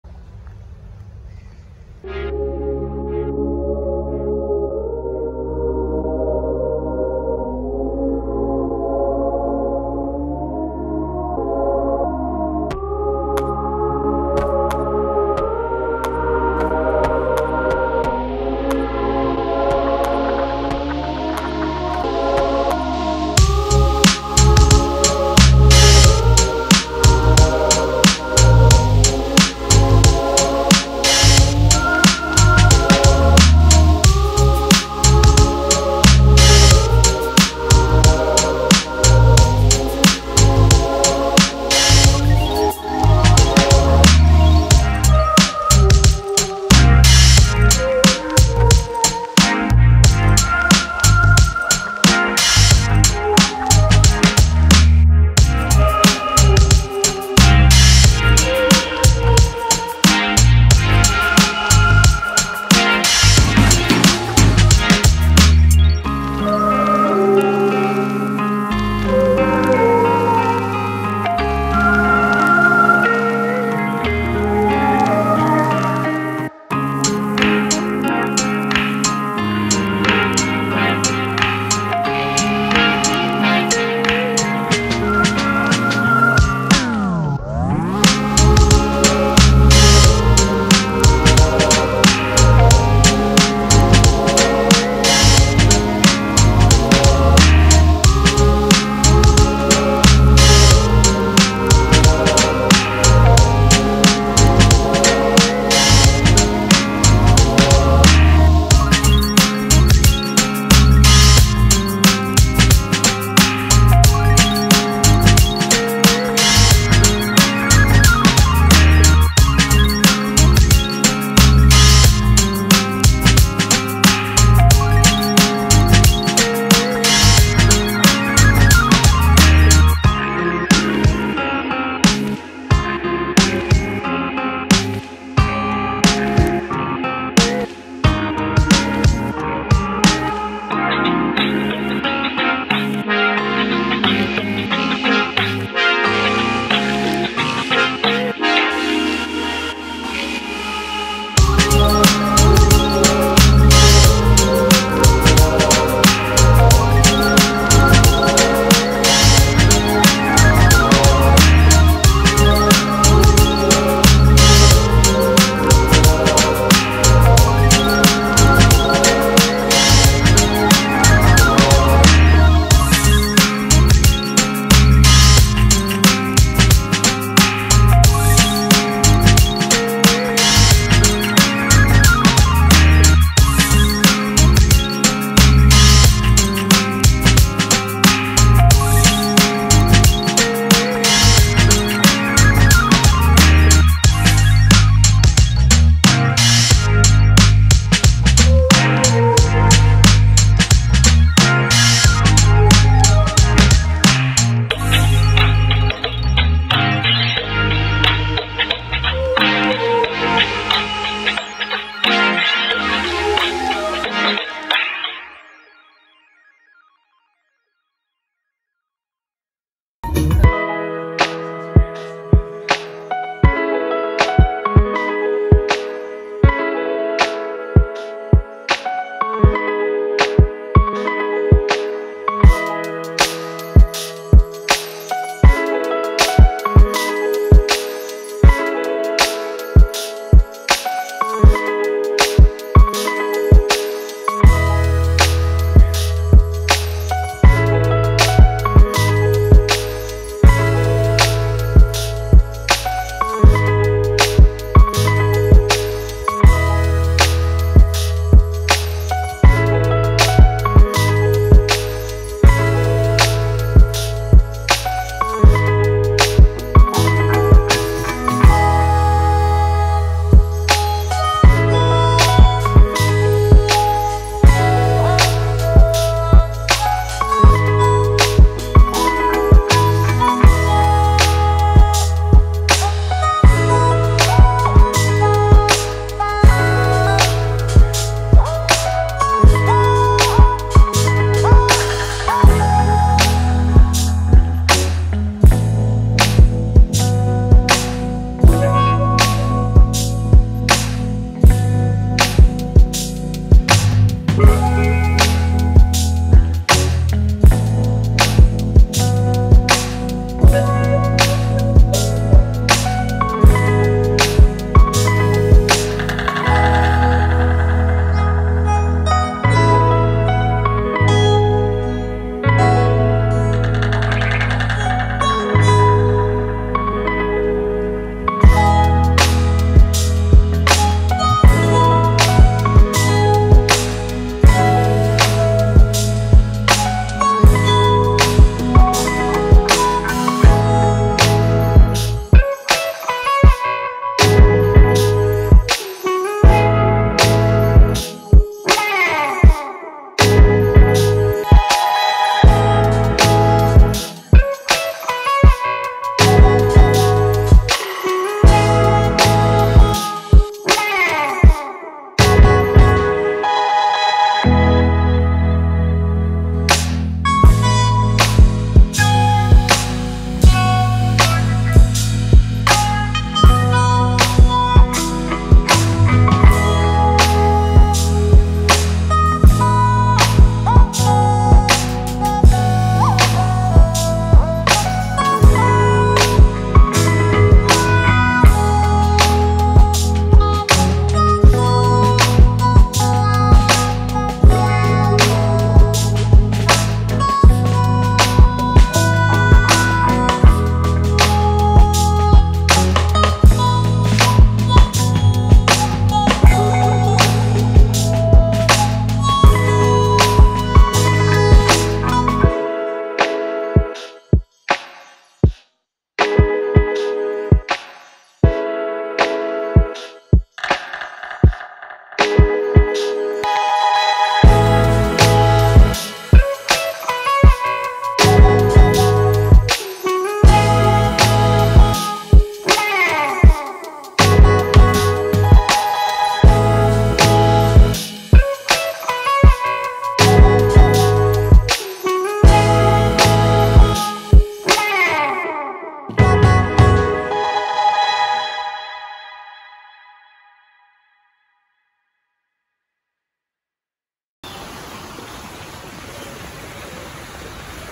7 days in DUBAI, Hotel Dukes the Palm + Safari tour in the desert
Dukes hotel the palm